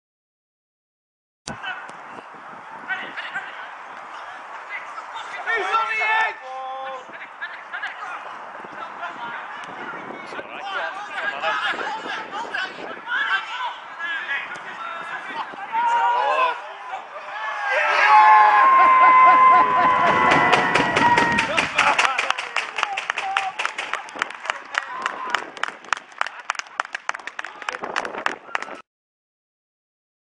Tom Hannigan flick header goal
Tom Hannigan leaps like a salmon to head home against Histon (2011-12).